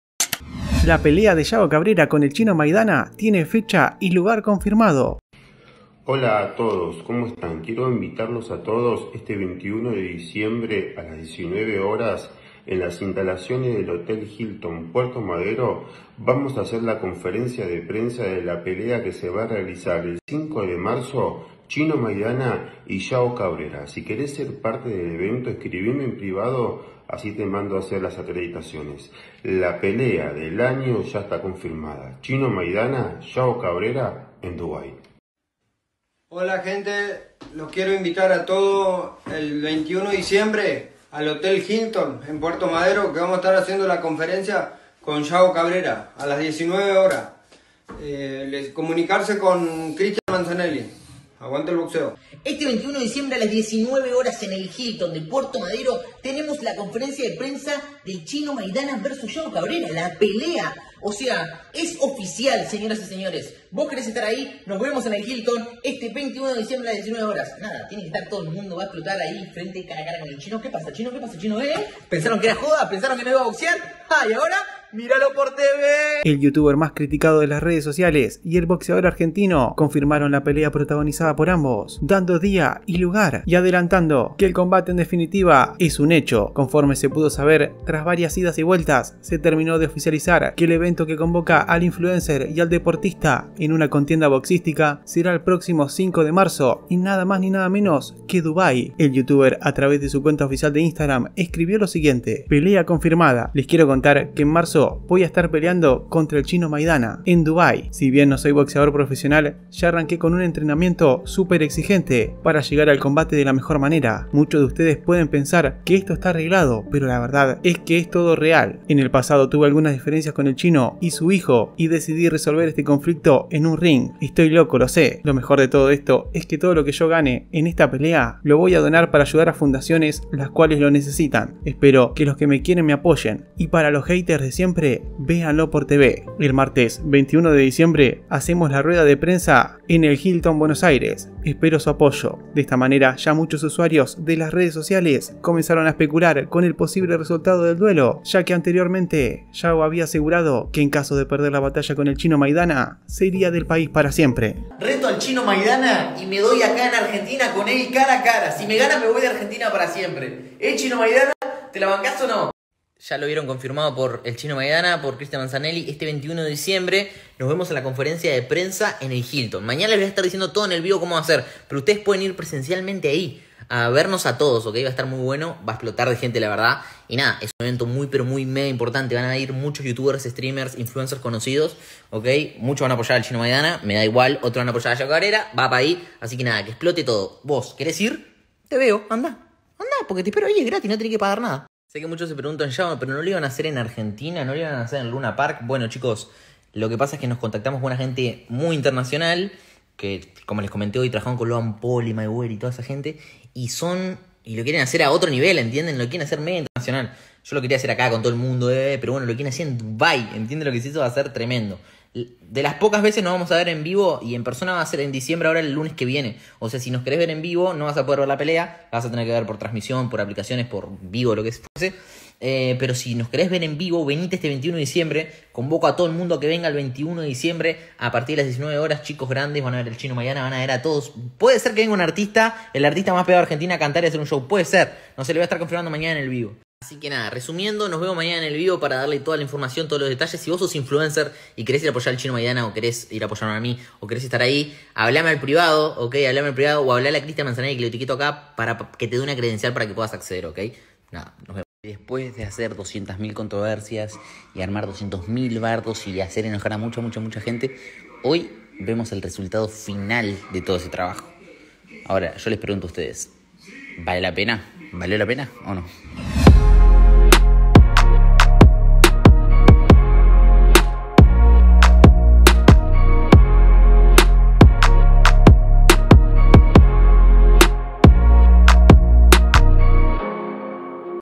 La PELEA de YAO CABRERA con el CHINO MAIDANA tiene FECHA y LUGAR CONFIRMADO
El youtuber más criticado de las redes sociales y el boxeador argentino confirmaron que la pelea protagonizada por ambos tiene día y lugar confirmado, dando cuenta de que el combate es un hecho.

Conforme se pudo saber tras varias idas y vueltas se terminó de oficializar que el evento que convoca al influencer y al deportista en una contienda boxística será el próximo 5 de marzo, en nada más ni nada menos que Dubai.

El youtuber a través de su cuenta oficial de Instagram escribió lo siguiente: “¡PELEA CONFIRMADA! Les quiero contar que en marzo voy a estar peleando contra chinomaidana.1 🥊 en DUBAI Si bien no soy un boxeador profesional, ya arranqué con un entrenamiento super exigente para llegar al combate de la mejor manera. Muchos de ustedes pueden pensar que esto está arreglado, pero la verdad es que es todo REAL. En el pasado tuve algunas diferencias con el Chino y su hijo y decidí resolver este conflicto en un ring( Estoy loco lo se ) Lo mejor de todo esto es que todo lo que yo gane en esta pelea lo voy a donar para ayudar a fundaciones las cuales lo necesitan. Espero que los que me quieren me apoyen y para los haters de siempre véanlo por TV. El martes 21 de Diciembre hacemos la rueda de prensa en el @hiltonbuenosaires espero su apoyo.”

De esta manera ya muchos usuarios de las redes comenzaron a especular con el posible resultado del duelo ya que anteriormente yao había asegurado que en caso de perder la batalla con el Chino Maidana se iría del país para siempre.